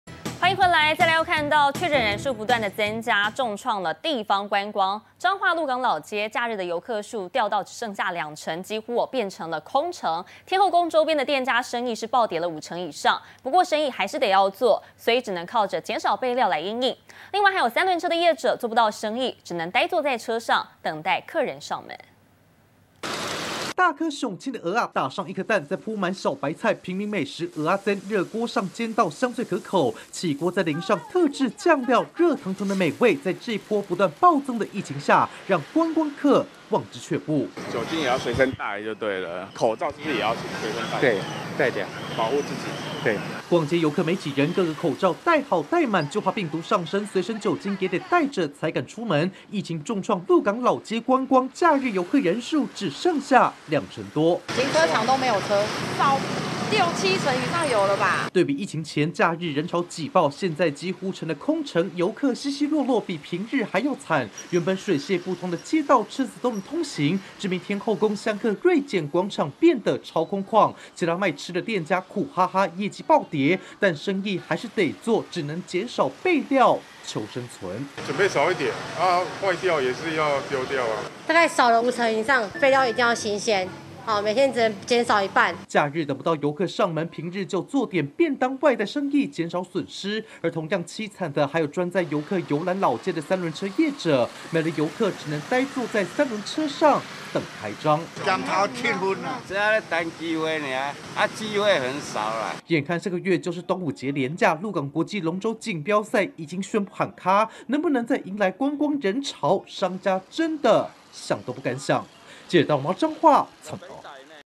疫情衝擊 鹿港小鎮變空城"遊客剩2成"
台灣走向與病毒共存，確診人數連日爆增，也重創地方觀光，彰化鹿港老街，假日遊客人數，掉到只剩兩成，幾乎變成空城，天后宮周邊店家生意，暴跌5成以上，不過生意還是得做，只能減少備料，求生存，另外還有三輪車業者做不到生意，只能呆坐在三輪車上，等待客人上門！